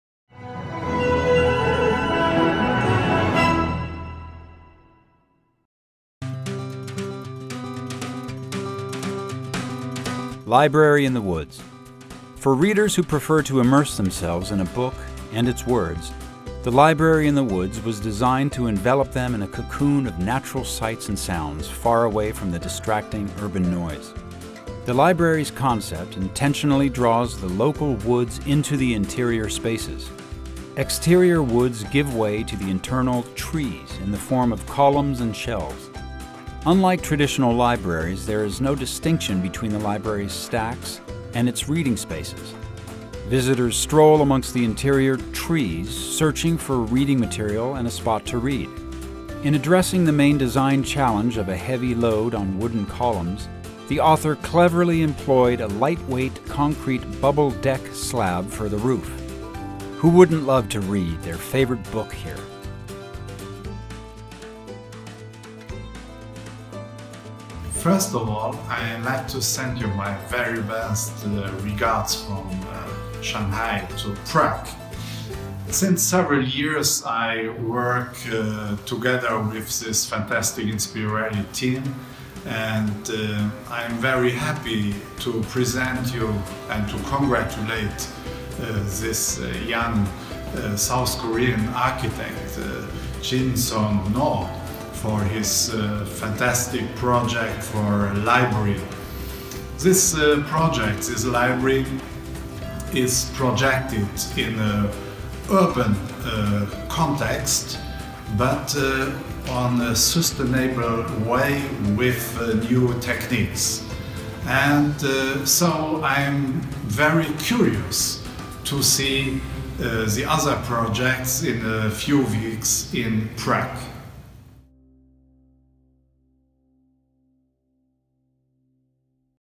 Library in the woods
Jinseon Noh (Republic of Korea), 2nd place in the Vision category, INSPIRELI AWARDS 3rd annual global contest.